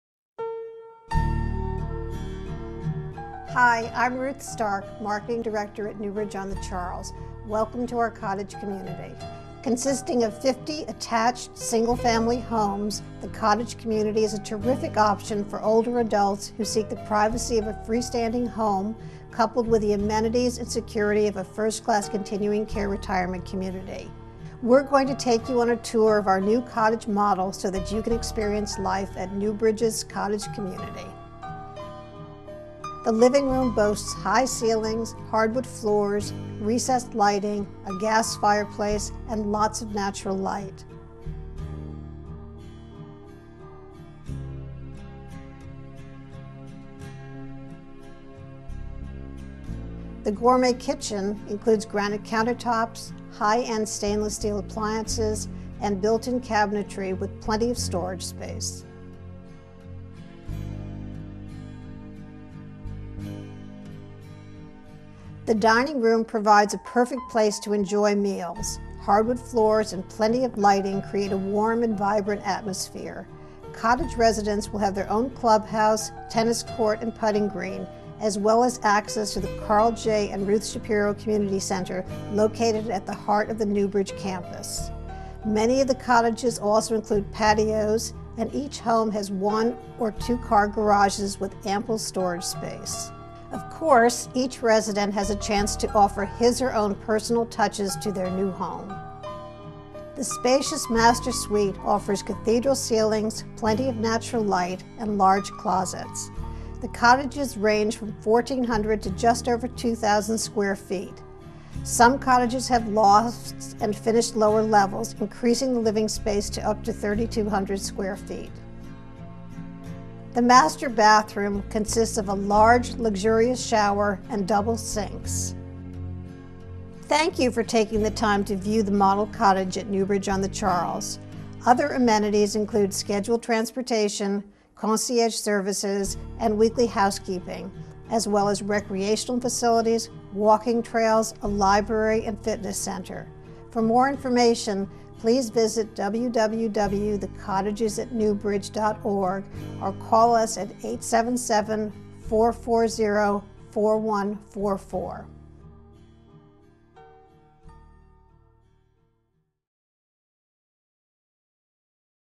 Tour NewBridge on the Charles' Model Cottage
The Cottage Community at NewBridge on the Charles consists of 50 attached, single family homes designed to provide residents with a sense of living in a traditional New England neighborhood.  Featuring a mix of traditional and contemporary furnishings, the model cottage highlights the units ample living space.